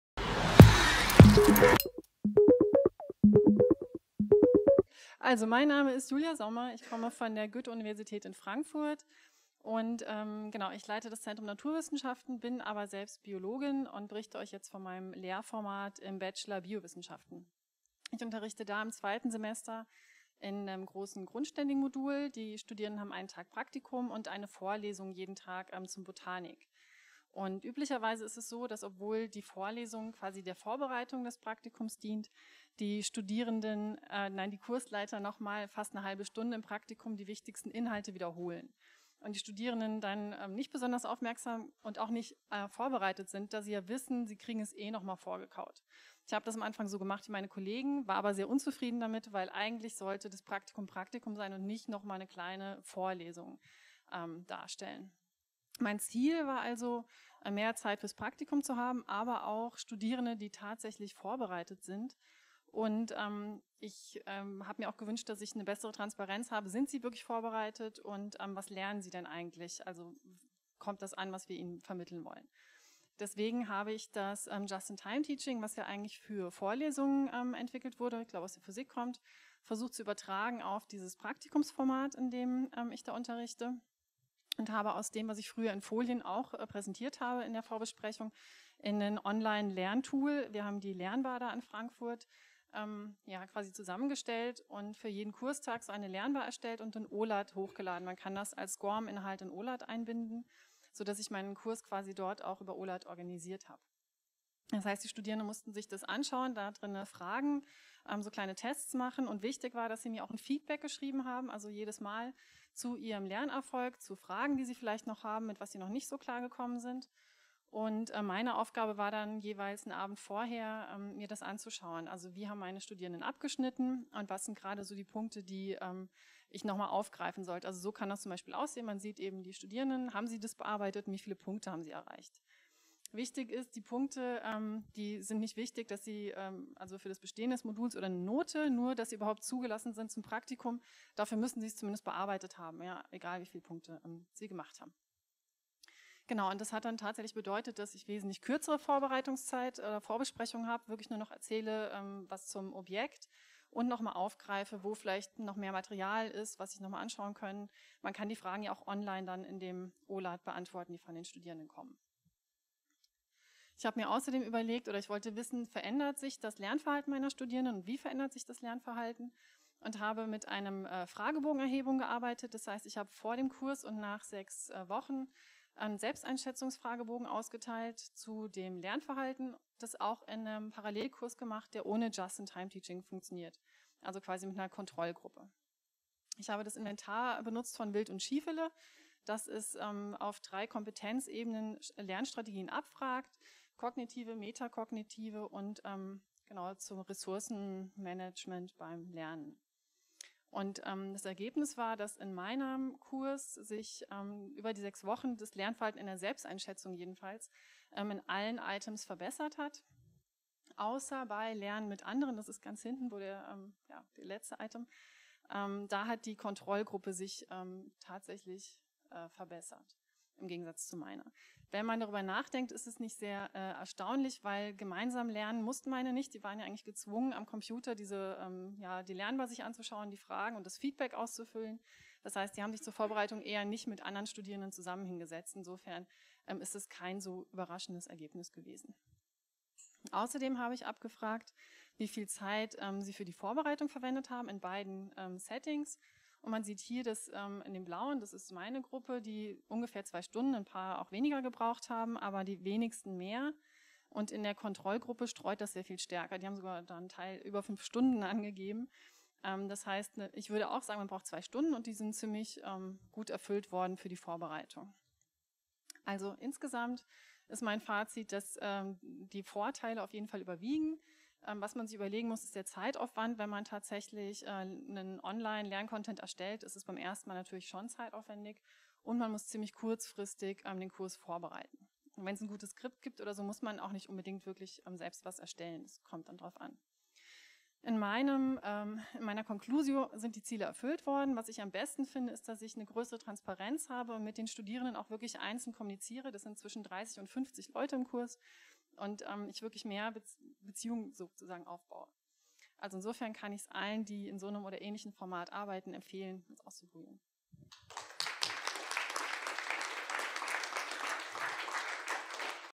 Einsatz von Just in Time Teaching - Julia Sommer
Dr. Julia Sommer stellt in ihrem Lightning Talk den Einsatz des Prinzips des Just in Time Teachings im Botanik-Praktikum vor. Dort ersetzt es die übliche theoretische Einführung durch die Kursleitung und stellt die Vorbereitung der Studierenden auf das Praktikum nahezu sicher. Darüber hinaus wurde wertvolle Praktikumszeit gewonnen. Eine Begleitstudie ergab, dass die Studierenden dieses Kurses ihre Lernstrategien im Vergleich zu einer Kontrollgruppe (ohne JiTT im Praktikum) verbessern konnten.